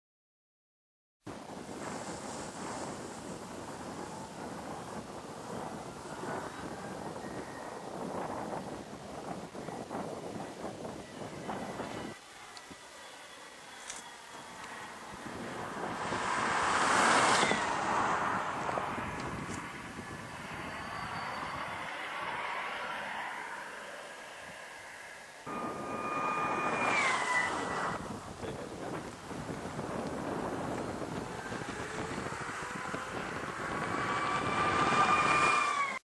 Volvo ReCharge WHEEL MOTOR Test Drive
The camera work is terrible but this is VERY rare footage of a wheel motor powered EV being driven at speed. The 'sound' is a result of the 16 khz carrier frequency used in the inverters.